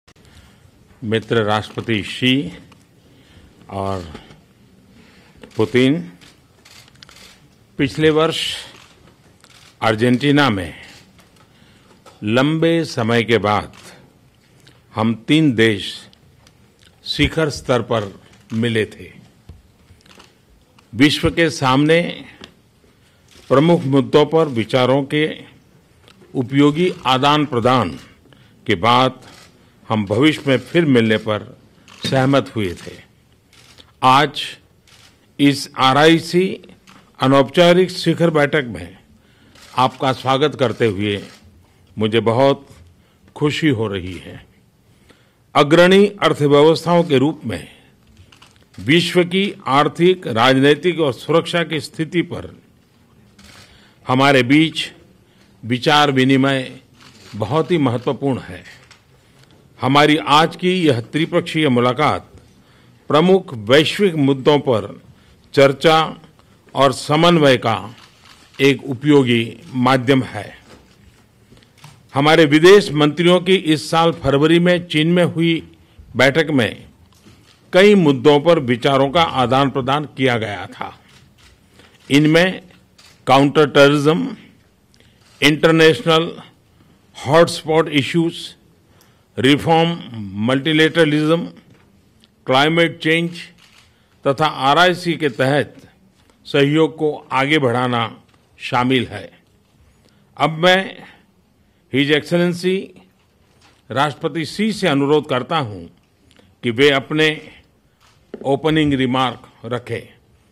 PM Modi's Speech | RIC (Russia, India, China) Leaders' Meeting
Prime Minister Narendra Modi on Friday 28 June chaired the Informal Meet of the RIC - Russia, India and China on the sidelines of the G20 Osaka Summit. The meeting was attended by Russian President Vladimir Putin and Chinese leader Xi Jinping. In his opening remarks Prime Minister listed terrorism, climate change and global recession as the three key challenges facing the world.